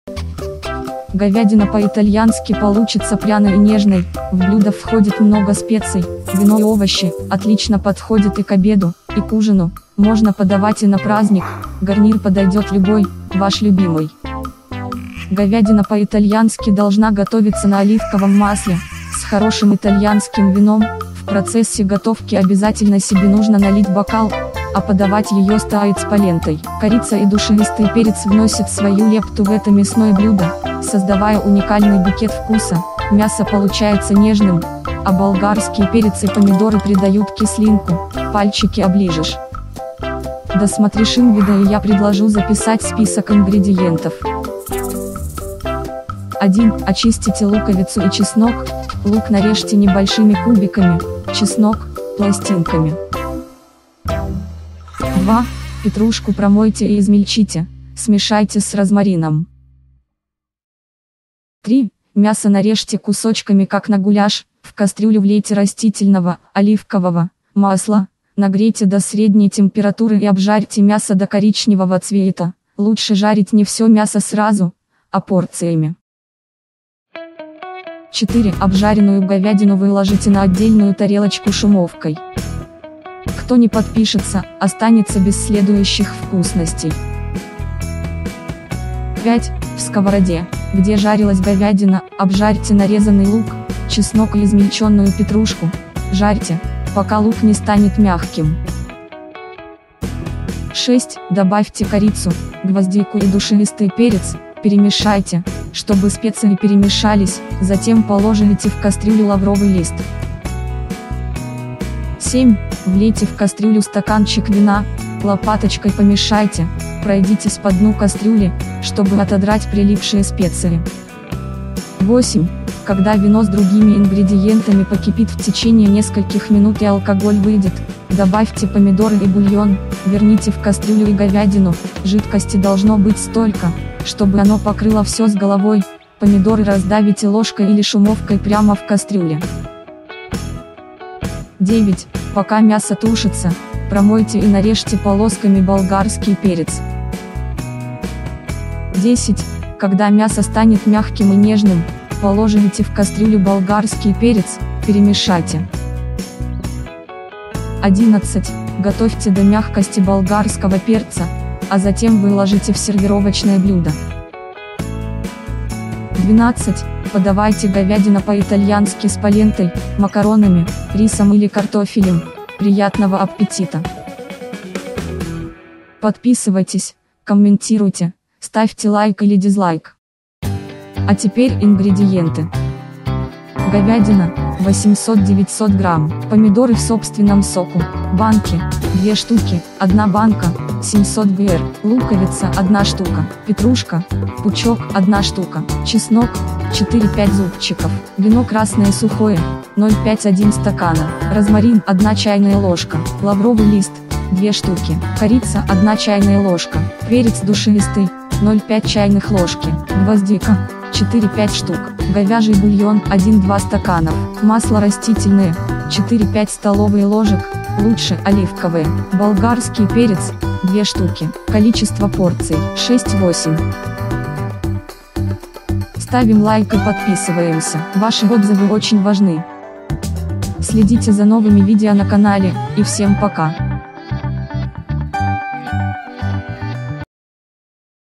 ★ ГОВЯДИНА ПО-ИТАЛЬЯНСКИ ★
★Новейший рецепт.★ Говядина по-итальянски получится пряной и нежной.★ В блюдо входит много специй, вино и овощи.★ Отлично подходит и к обеду, и к ужину.★ Можно подавать и на праздник.★ Гарнир подойдет любой, ваш любимый.★